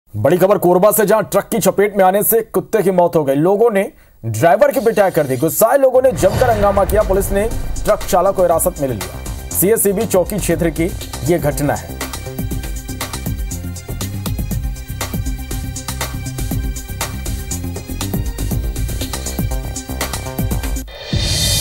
Korba Accident News : Truck की चपेट में आया कुत्ता | लोगों ने की ड्राइवर की पिटाई
IBC24, Korba Accident News , Truck की चपेट में आया कुत्ता, लोगों ने की ड्राइवर की पिटाई